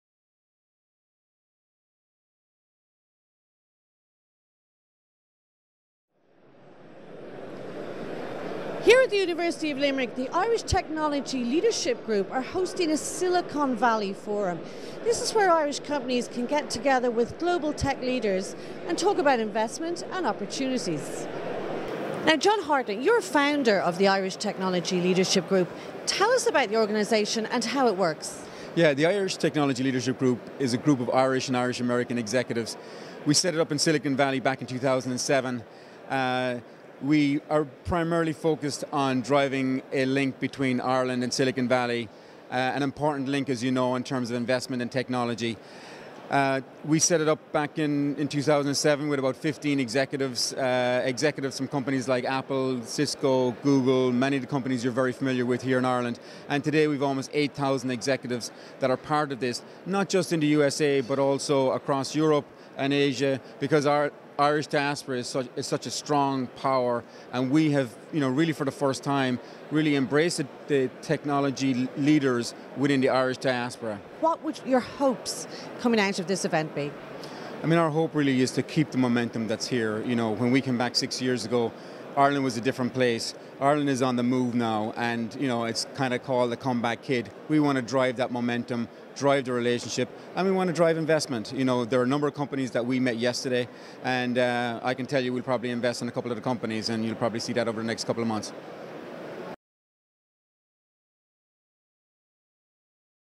John Hartnett at ITLG 2014
President and Founder of ITLG John Hartnett, discusses the Group's role in driving investment and technology innovation in Ireland.